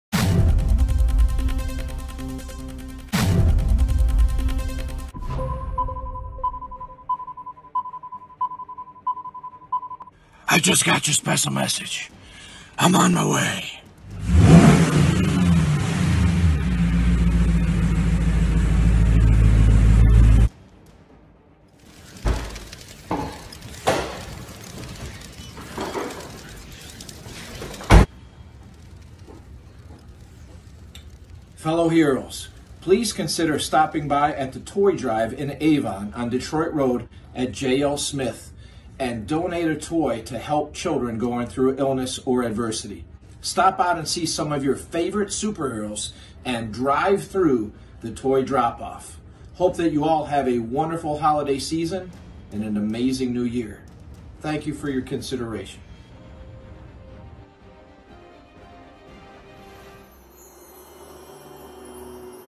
TOY DRIVE-THROUGH & DROP 12/11/2021 w SUPER HEROES TO KIDS IN OHIO at 36610 Detroit Rd in Avon, Ohio
MEET SOME OF YOUR FAVORITE SUPER HEROES at THE TOY DROP & DRIVE THROUGH on Saturday, December 11, 2021! Super Heroes To Kids In Ohio is having a drive- through toy drive on Saturday, December 11th at JLSmith Group at 36610 Detroit Rd in Avon, Ohio. The time will be Noon until 3pm! Thank you so very much JL Smith! Make sure to bring your cameras for some great photo ops!!! ***CAN'T MAKE IT? Feel free to drop off any new toys at their Avon office up to the event. THANK YOU SO VERY MUCH FOR HELPING US GET TOYS AND GOODIES TO KIDS WHO CAN USE A BOOST THROUGHOUT THE YEAR! New and unwrapped Super Hero and Princess toys are cherished, but we will absolutely take any new / unwrapped toys of any kind!